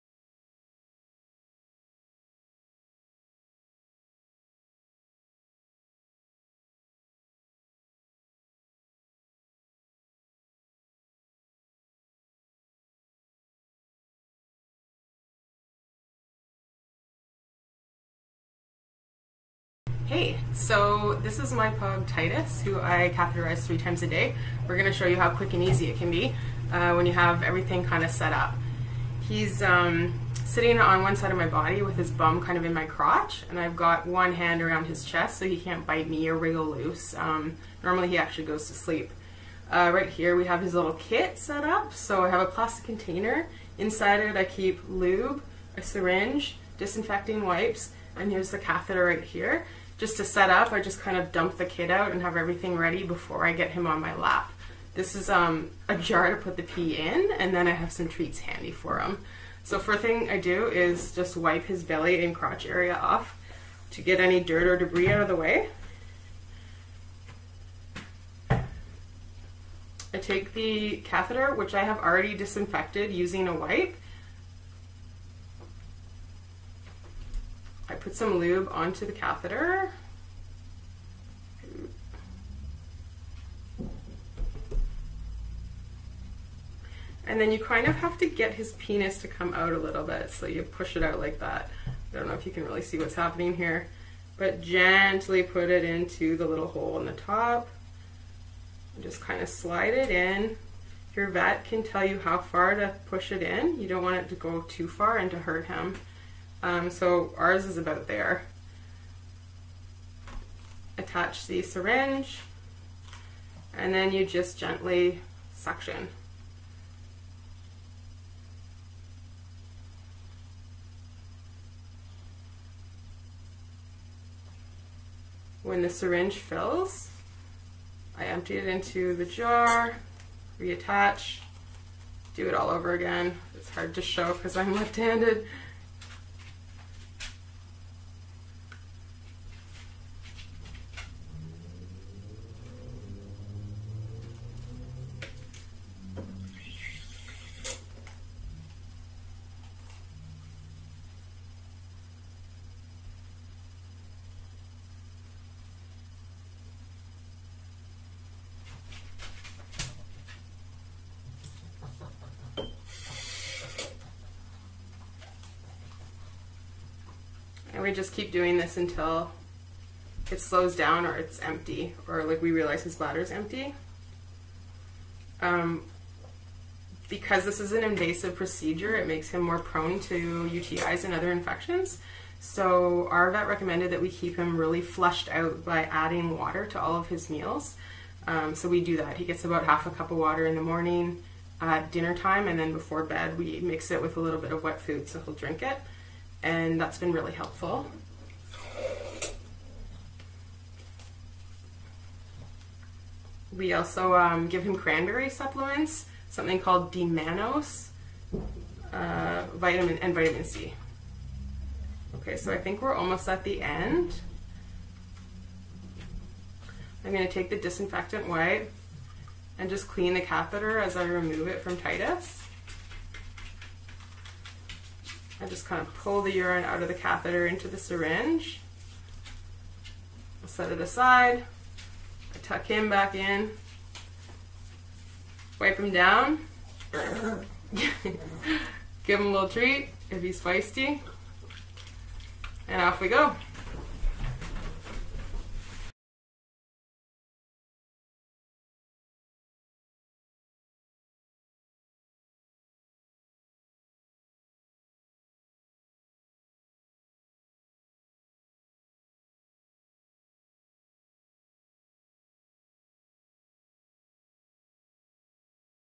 Home Catheterization with Titus the Pug
This is shared as a personal story and is not veterinary/medical advice.

Titus has been catheterized for 4 years, following diminished mobility and reduced function of his hind-quarters - resulting from Pug Myelopathy. He lost the ability to urinate independently. This is believed to be a neurological effect of his degenerative spinal disease.

After an awkward and stressful first several weeks, and under careful veterinary instruction and support, we've narrowed down practical tools and fine-tuned a system that helps things run pretty smoothly for us by this stage.

Sorry about the crappy video editing program and low production value! Had to use what I had on hand.

Other notes:

- Titus is not able to be manually expressed, a method that is preferable to catheterization and less invasive for emptying the bladder of urine, when a dog loses this critical function. (Manual expression also requires vet recommendation/instruction.)

- Ideally, catheters and syringes are single-use items. Due to the full-time, long-term nature of Titus' home catheterization, we elect to re-use these supplies, by carefully cleaning them with medical grade disinfecting wipes. (Note: These are NOT Lysol wipes.)